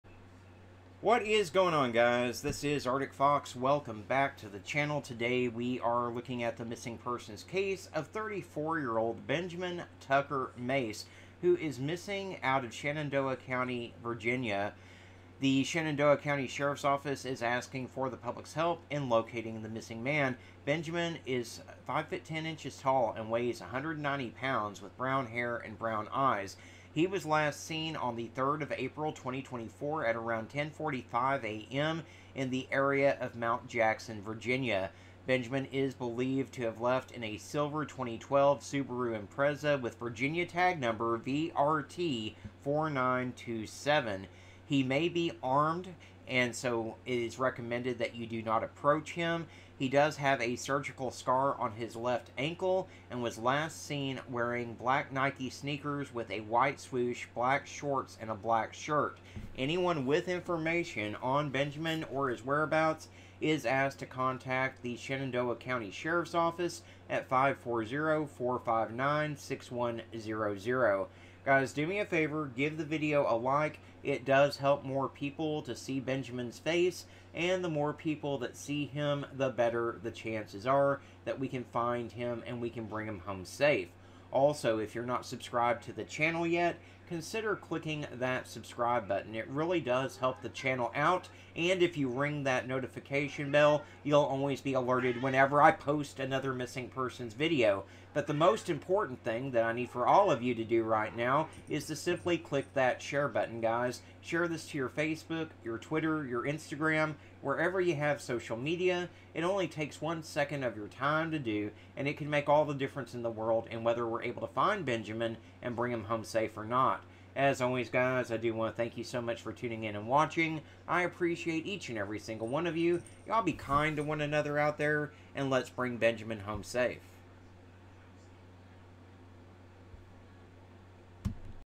34 YEAR OLD BENJAMIN MACE IS MISSING FROM MT JACKSON VIRGINIA. SHARE HIS FACE TO BRING HIM HOME!!!
ALCOHOLICS ANONYMOUS